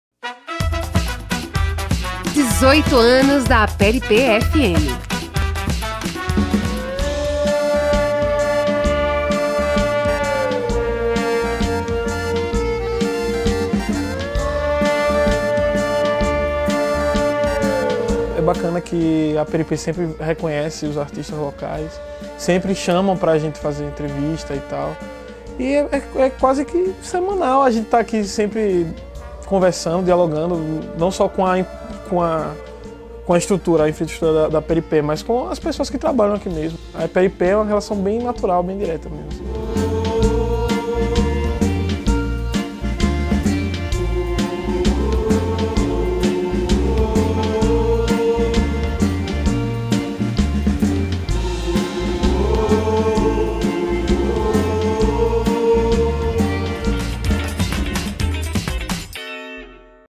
APERIPÊ FM - COUTTO ORCHESTRA
Depoimento de Vinicius, da Coutto Orchestra, para o aniversário de 18 anos da Aperipê FM 104,9.